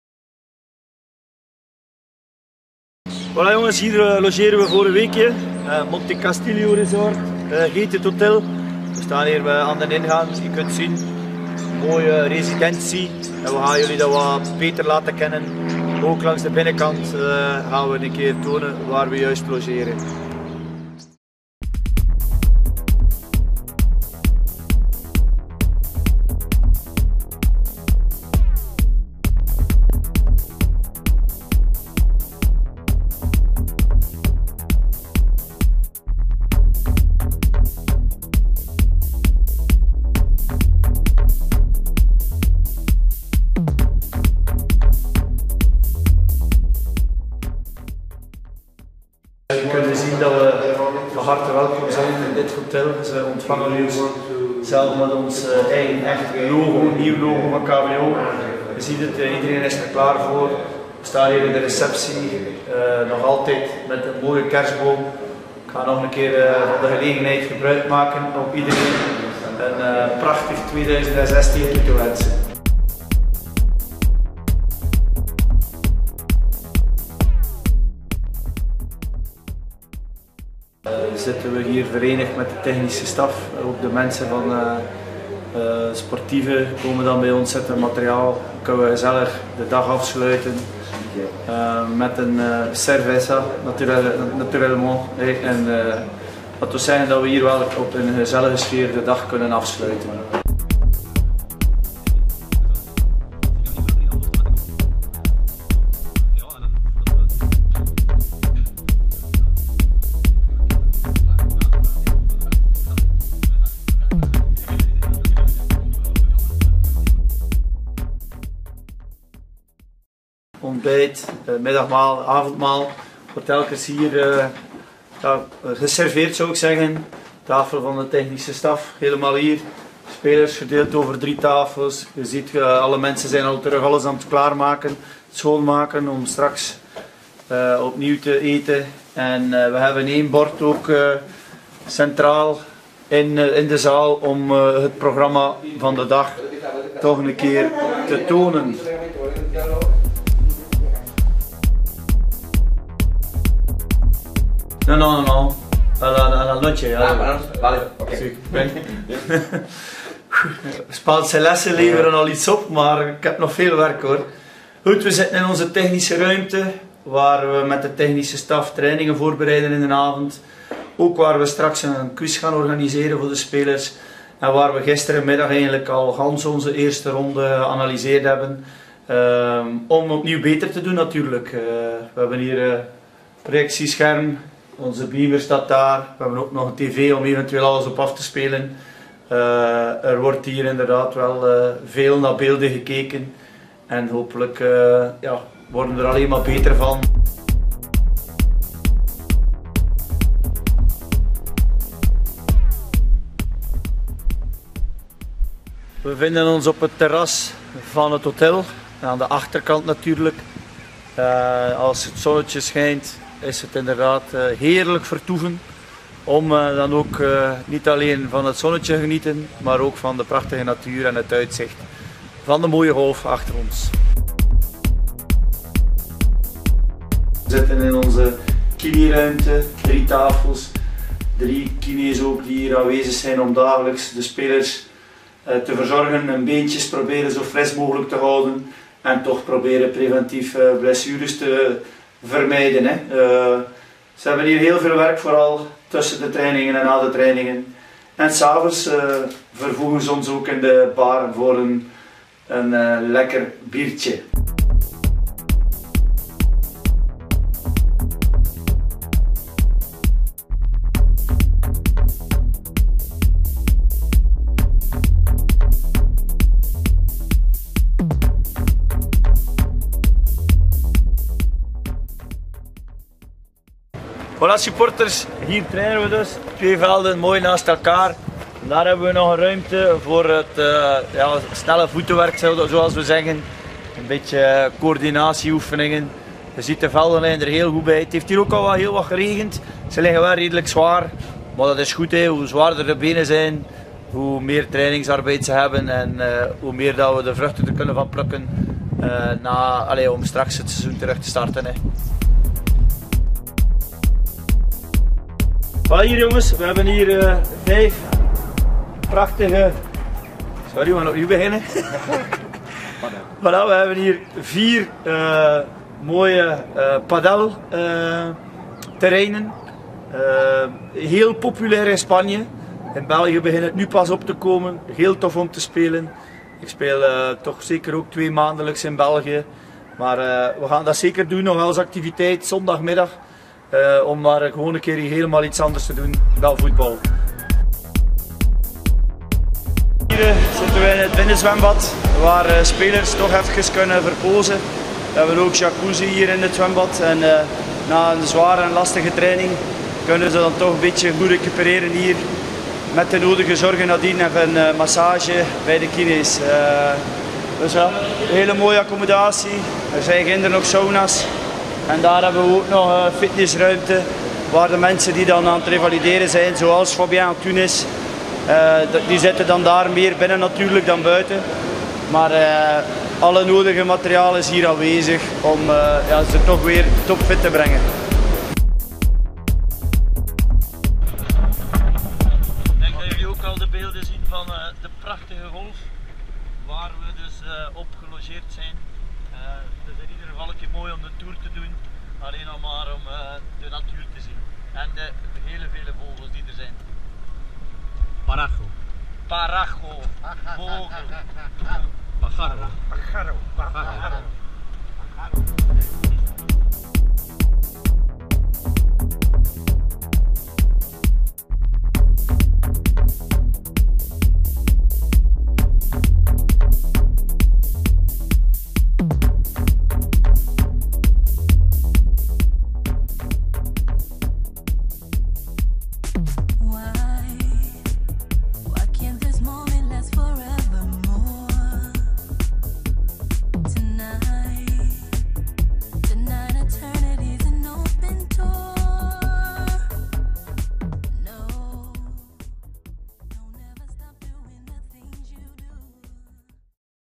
KV Oostende winterstage - rondleiding hotel Barcelo in Jerez
Met Yves Vanderhaeghe als professionele tour guide.